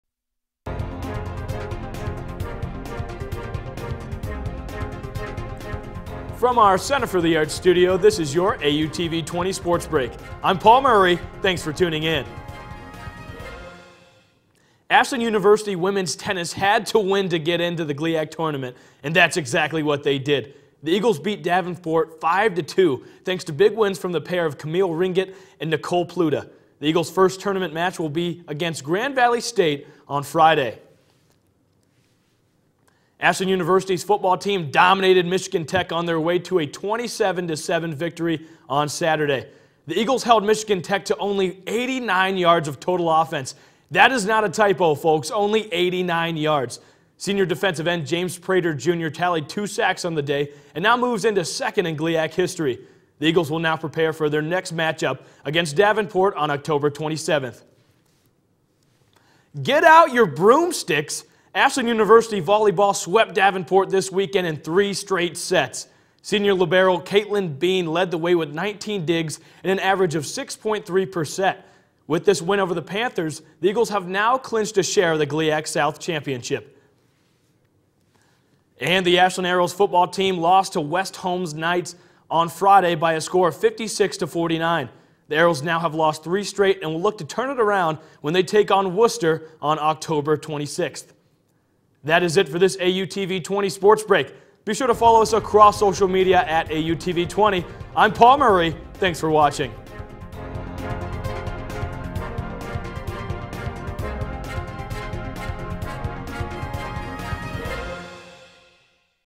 10-22-18 Sports Break
Join us for today's exciting edition of AUTV-20's Sports Break, hosted by executive news producer, Paul Murray. This production was directed by Zach Read.